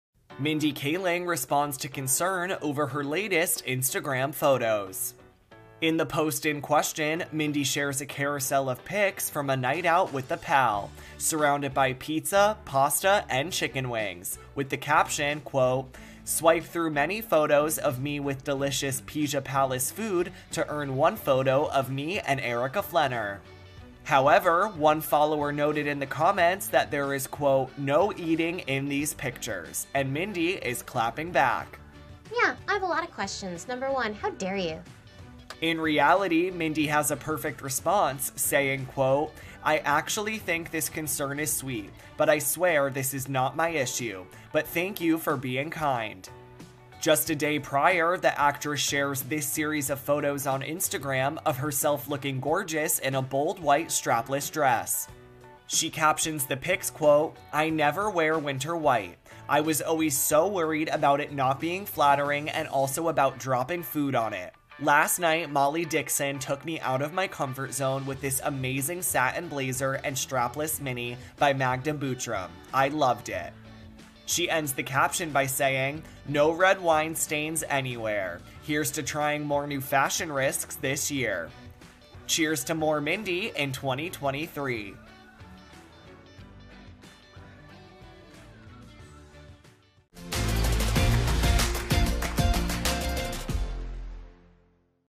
Mindy Kaling Dishes Perfect Clapback to Concern Over Food Photos | E! News
Mindy Kaling responds after a commenter expressed concern over her eating habits in a recent Instagram post. See her flawless clapback!